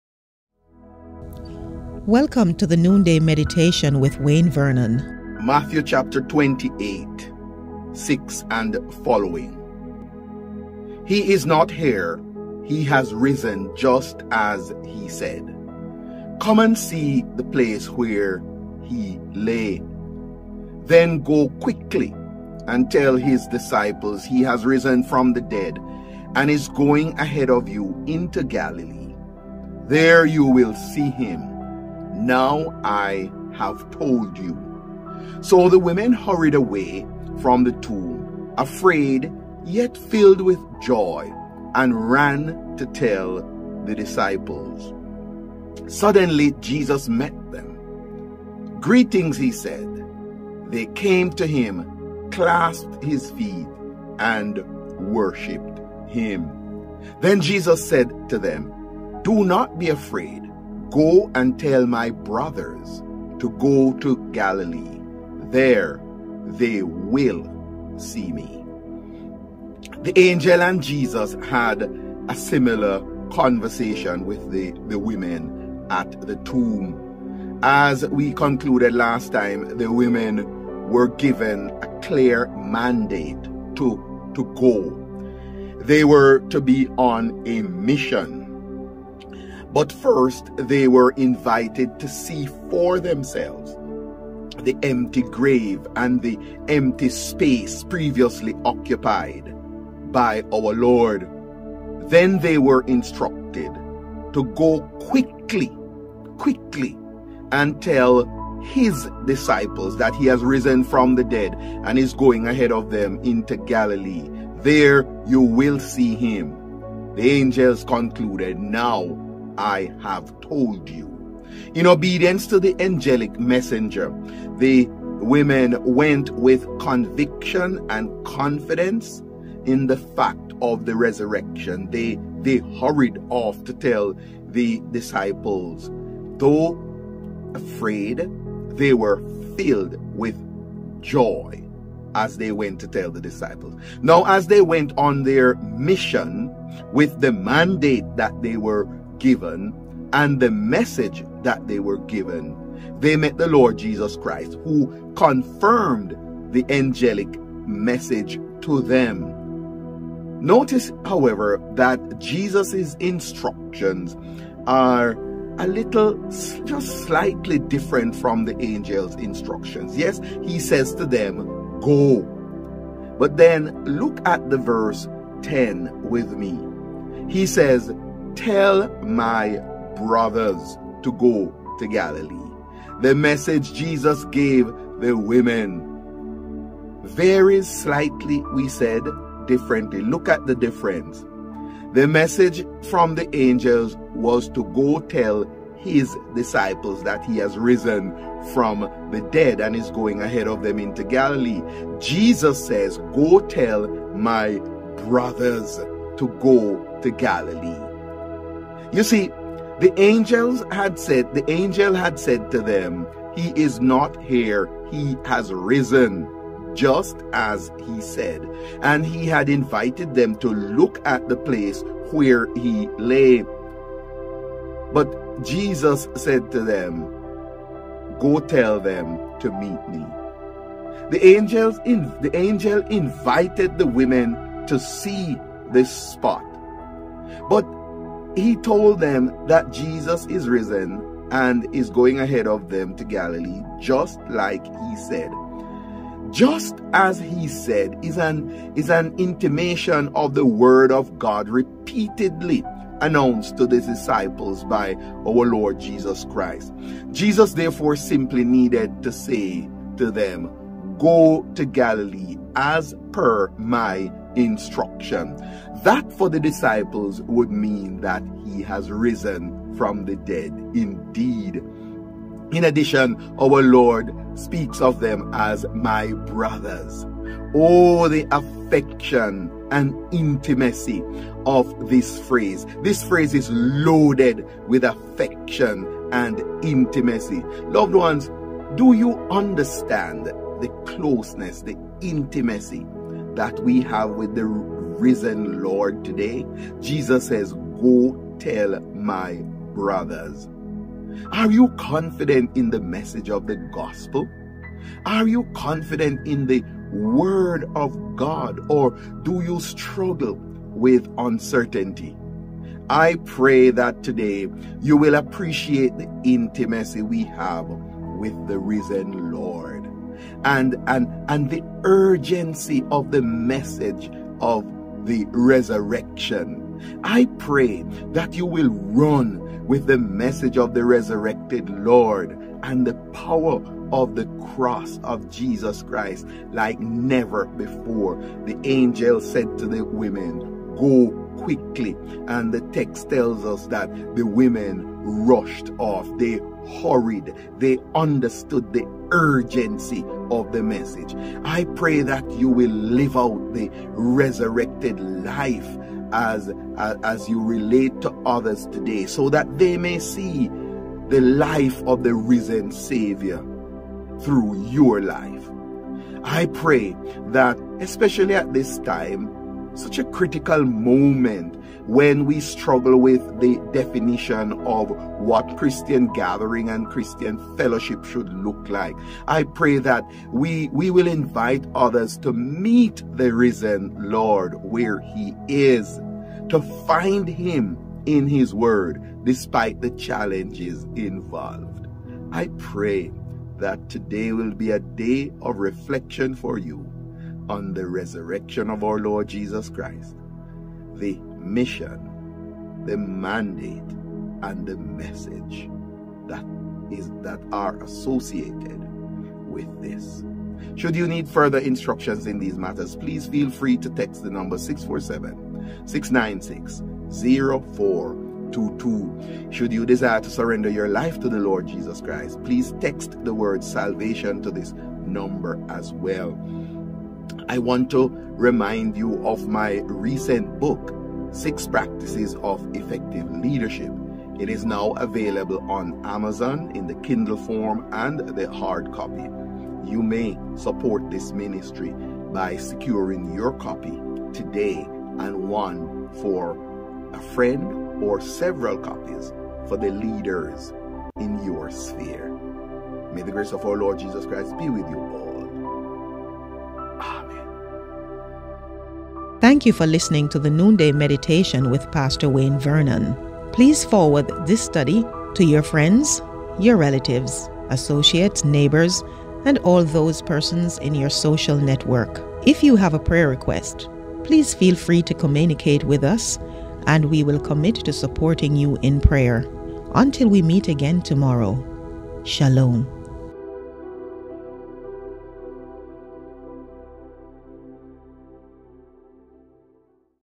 Post Resurrection Conversations NDM April 21, 2022
Noonday Meditation with Wayne Vernon for April 21, 2022